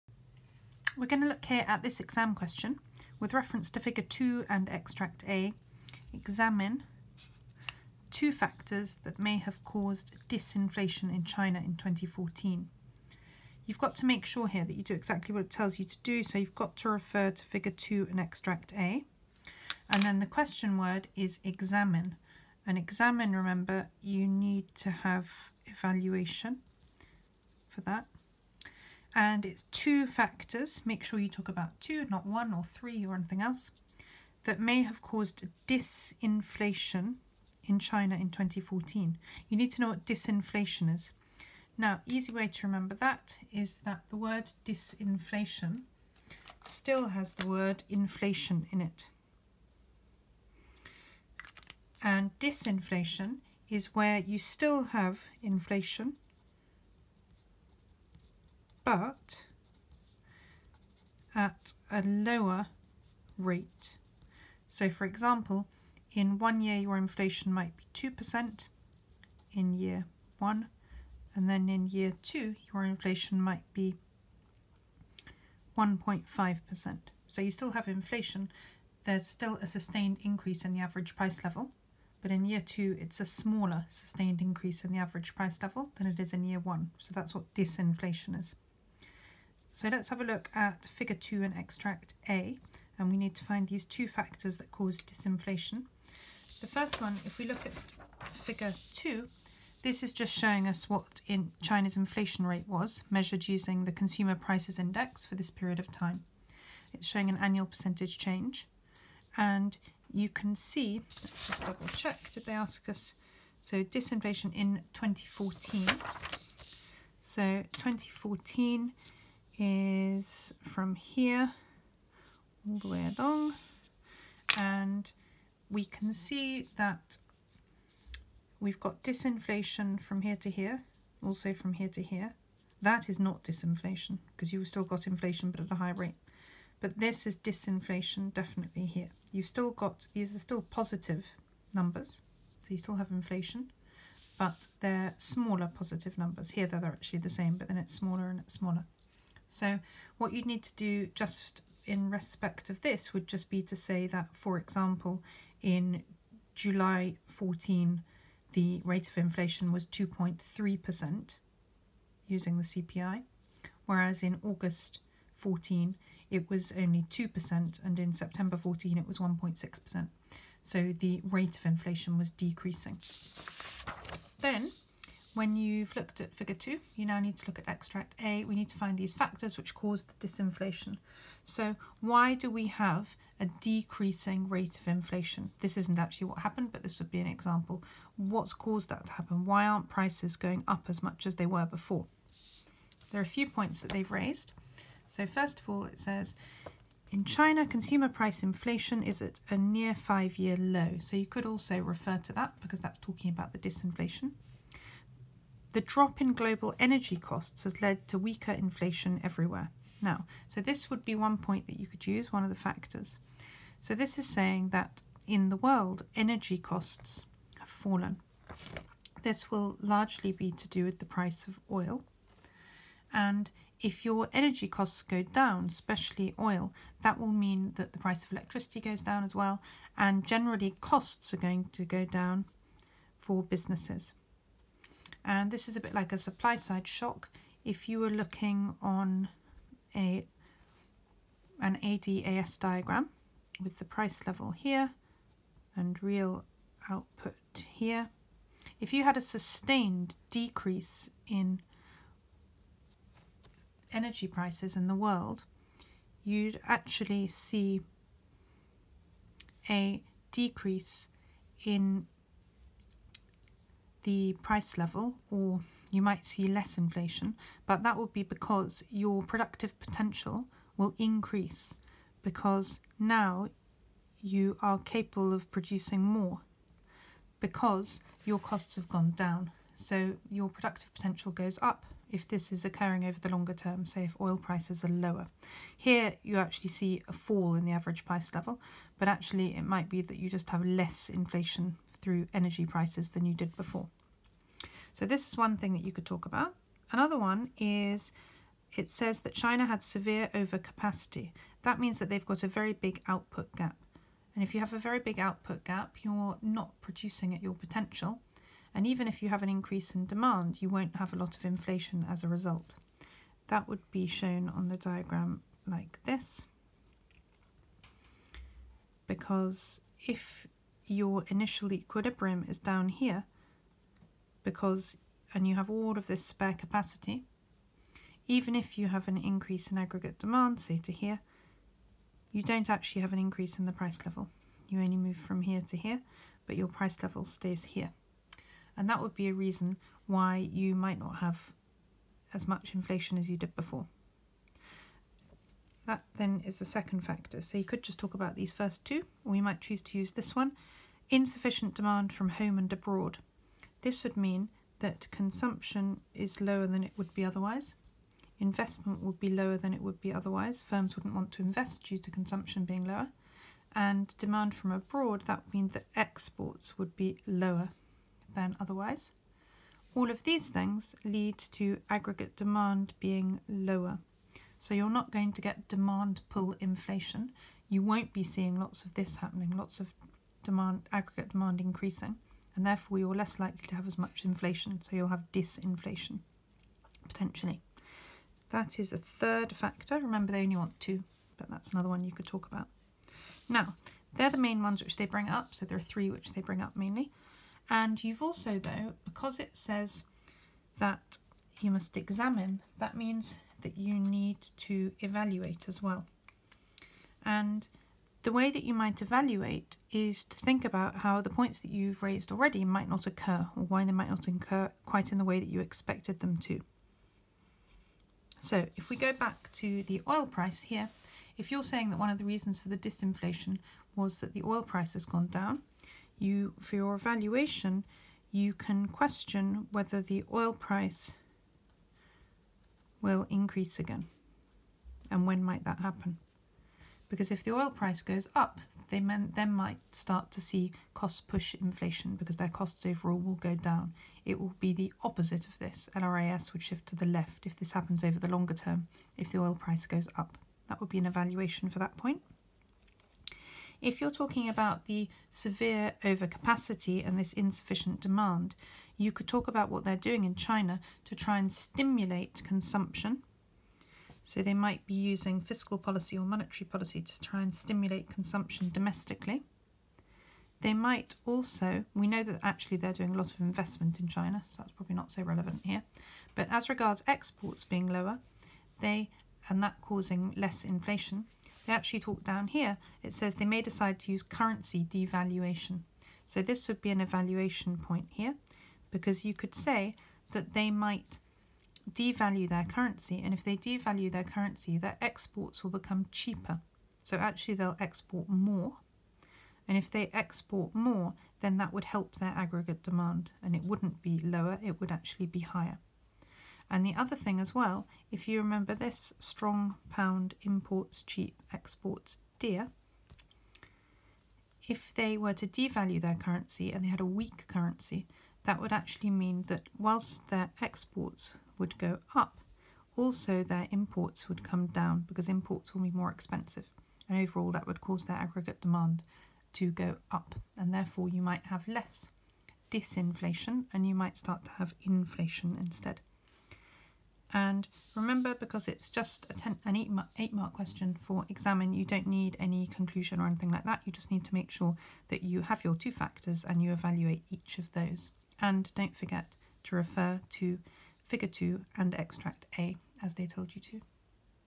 Causes of disinflation in China - 8 mark ‘examine’ question
Explaining how to answer this 8 mark exam question, including: the definition of disinflation, interpreting a bar chart showing inflation data, how to extract pieces from data, structuring your answer and writing evaluative points.